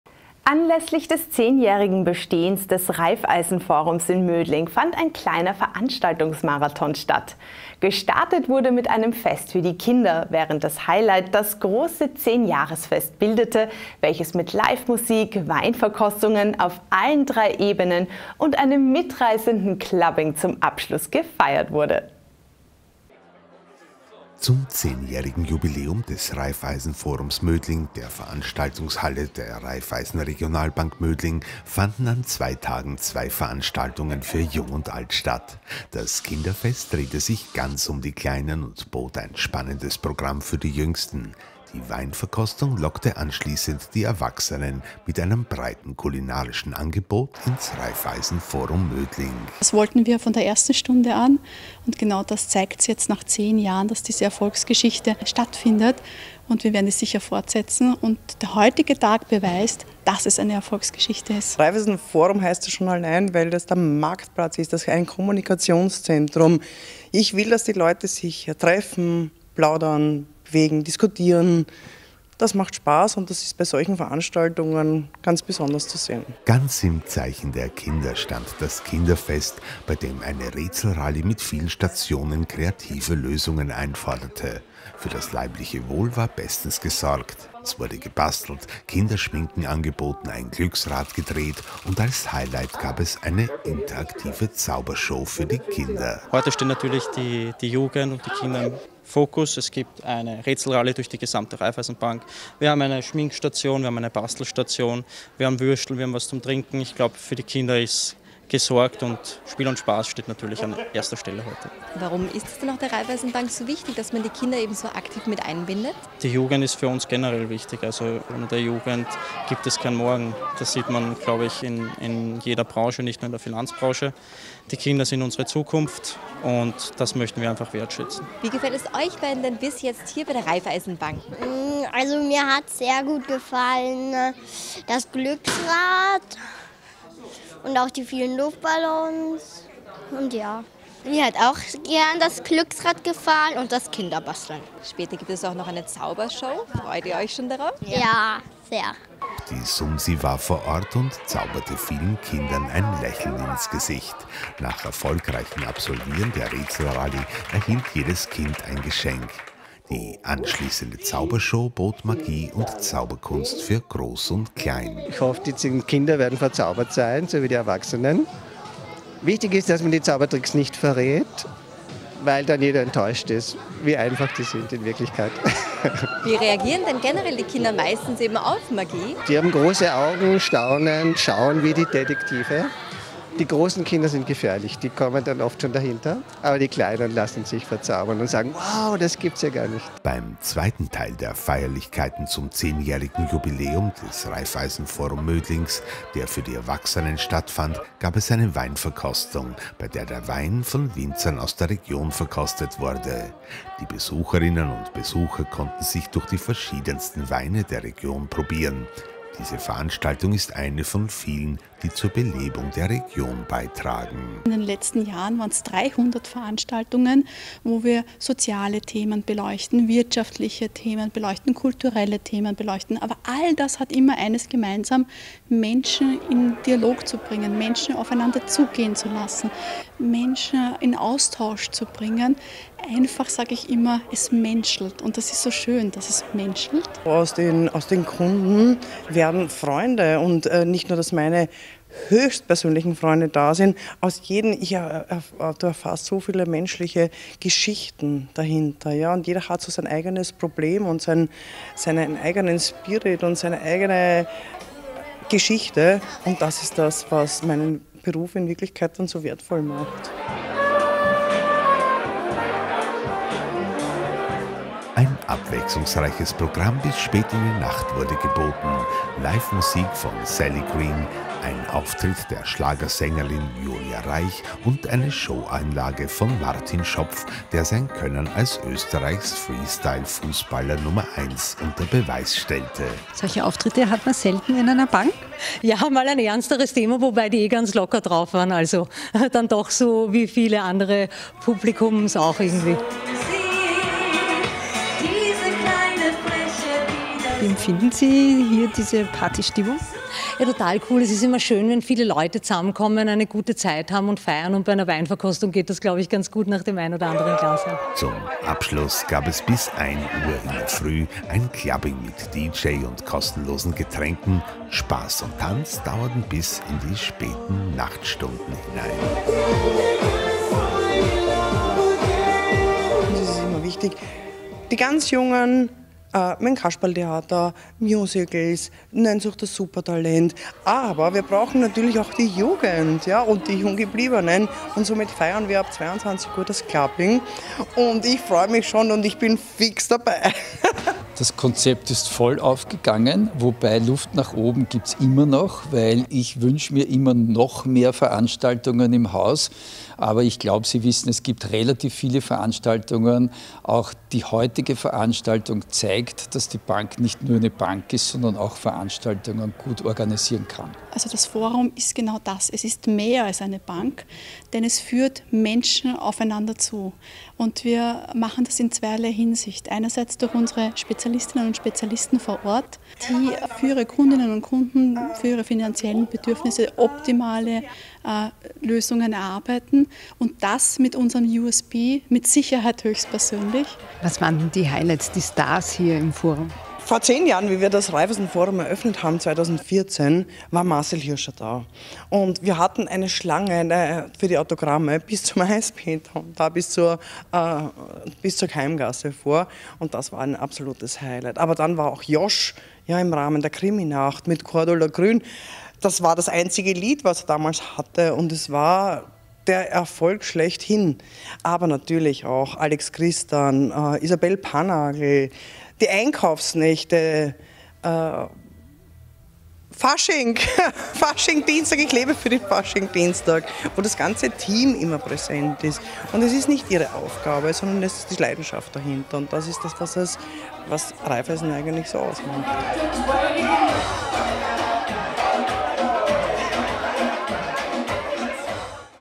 10 Jahre Raiffeisen-Forum Mödling
Anlässlich des 10-jährigen Bestehens des Raiffeisen-Forums in Mödling fand ein kleiner Veranstaltungs-Marathon statt. Gestartet wurde mit einem Fest für die Kinder, während das Highlight das große 10-Jahresfest bildete, welches mit Live-Musik, Weinverkostungen auf allen drei Ebenen und einem mitreißenden Clubbing zum Abschluss gefeiert wurde.